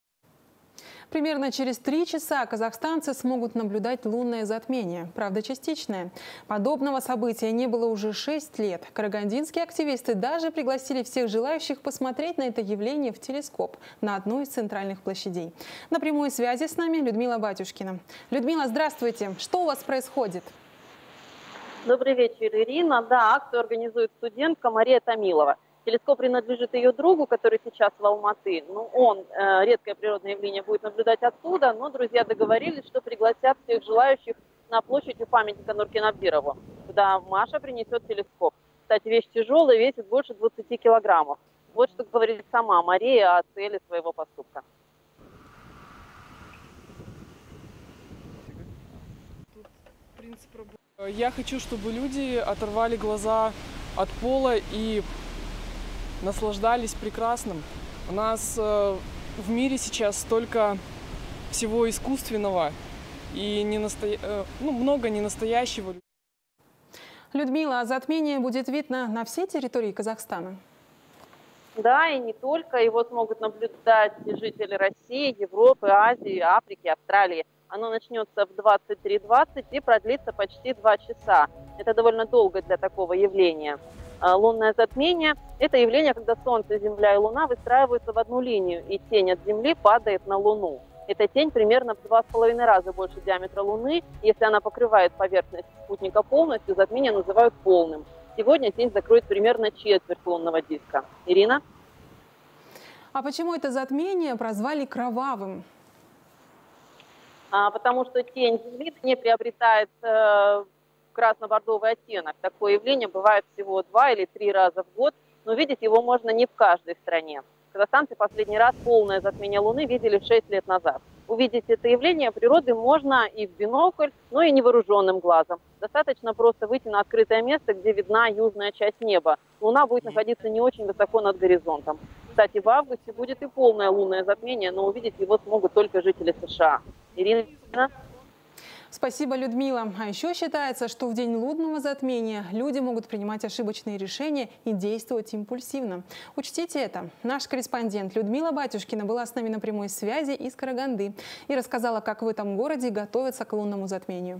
Казахстанцы смогут наблюдать первое за шесть лет лунное затмение
Примерно через три часа казахстанцы смогут наблюдать лунное затмение, правда, частичное. Подобного события не было уже шесть лет. Карагандинские активисты даже пригласили всех желающих посмотреть на это явление в телескоп на одной из центральных площадей.